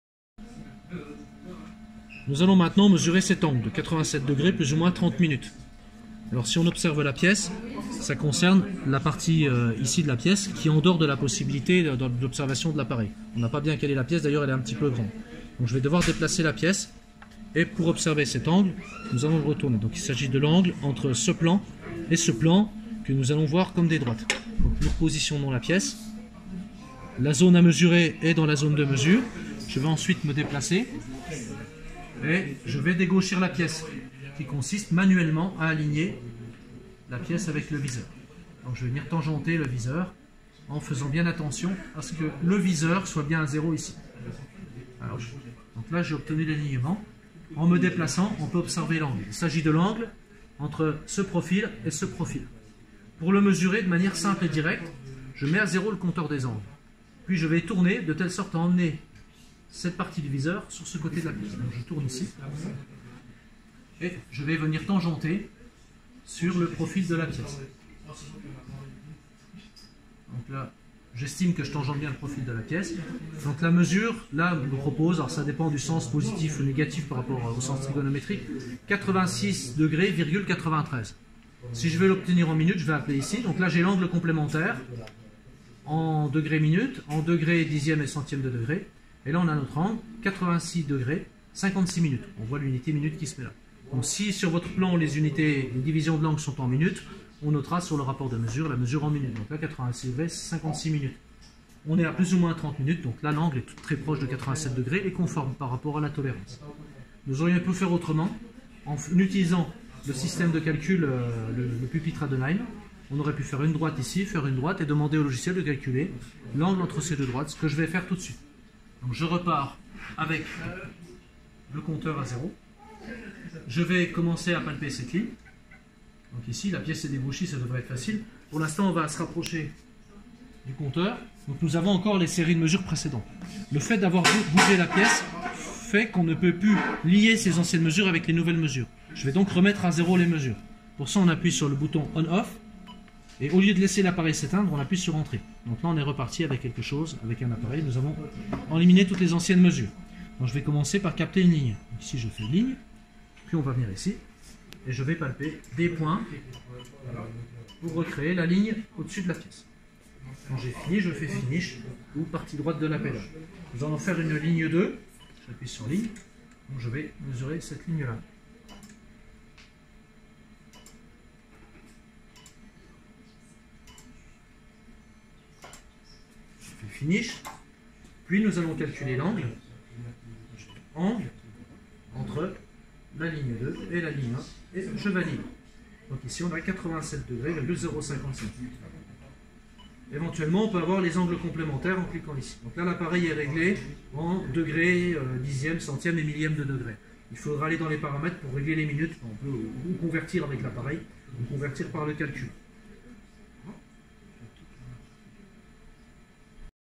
projecteur5
TP utilisation du projecteur de profil IUT Nancy-Brabois département QLIO à Lunéville